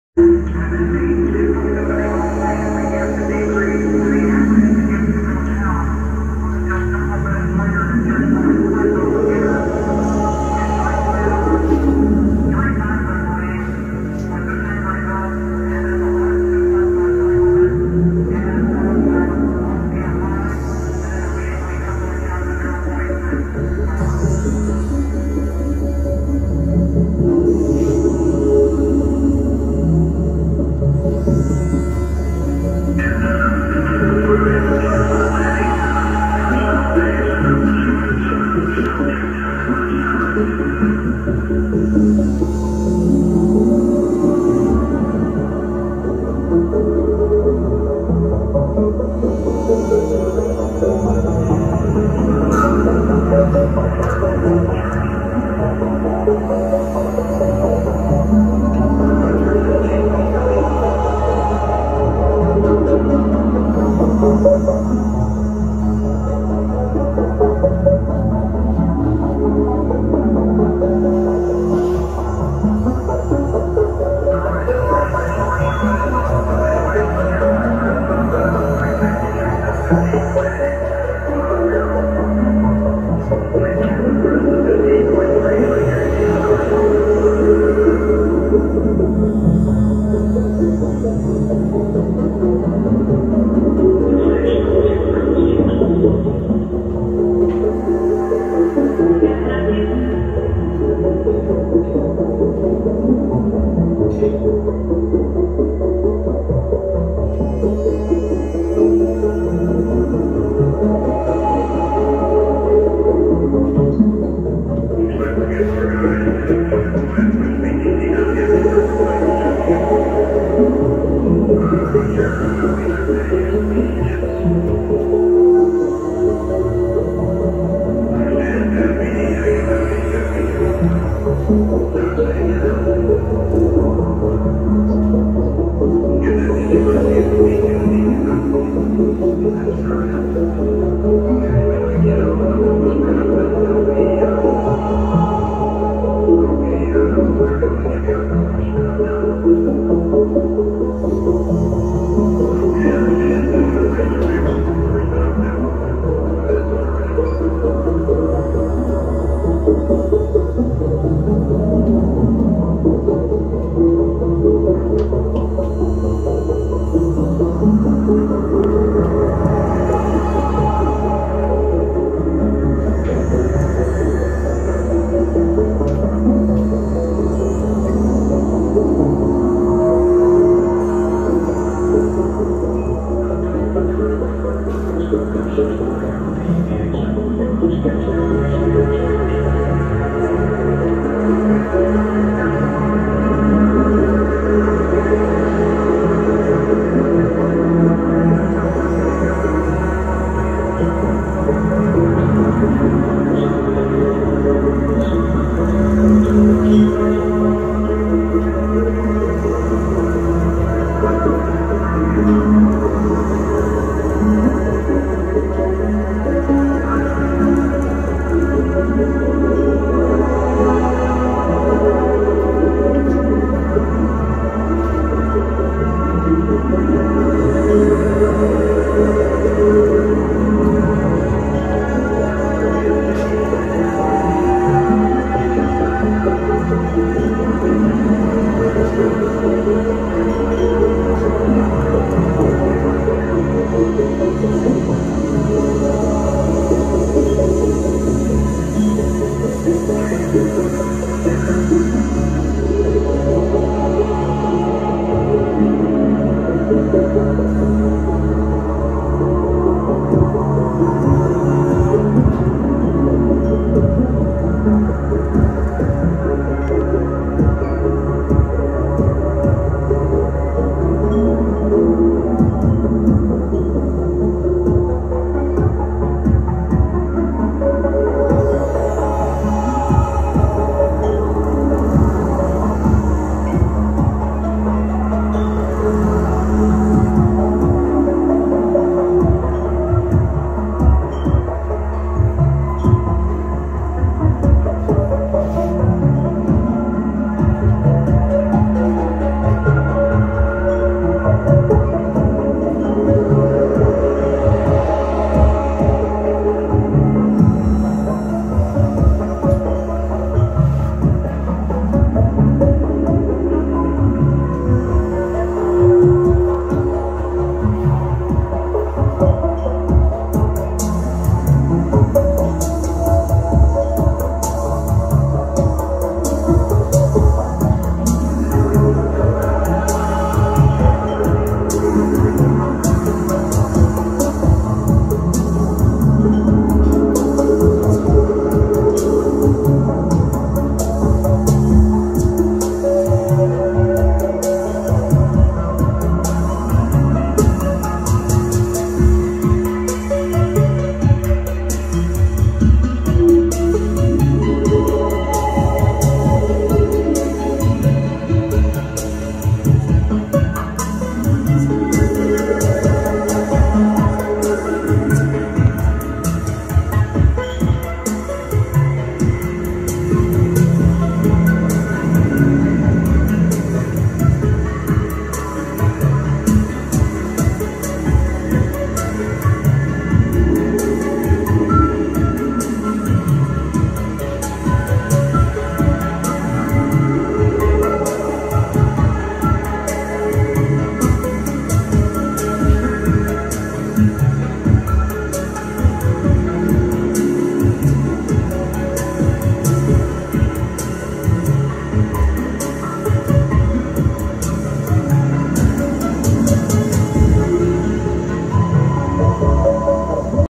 Tectonia Live @ The Fortress - July 2nd, 2023 (excerpt)
Here’s a little piece of the live set I did on July 2nd, 2023. It was an honour to play at such a nice location. I enjoyed it very much.